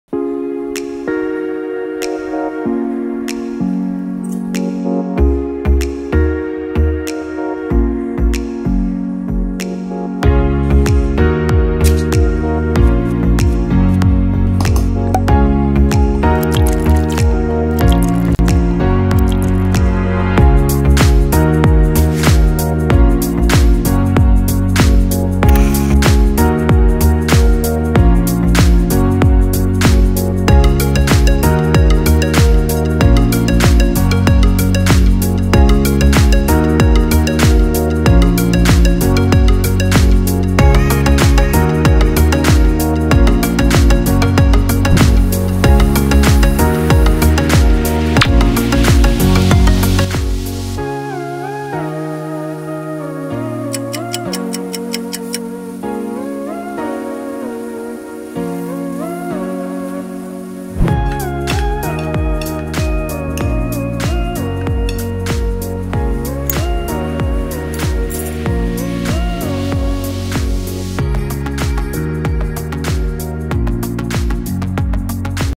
Thai Style - Stuffed minced beef Sotong
Must have this with Thai Seafood dipping sauce! It's so delicious and refreshing at the same time.

Ingredients:
5 tbsp Freshexpress Sg Green Curry paste
300g minced beef
5 tbsp fish sauce
1/2 tsp salt (can be 1 tsp up to your preference)
1 stalk of chopped coriander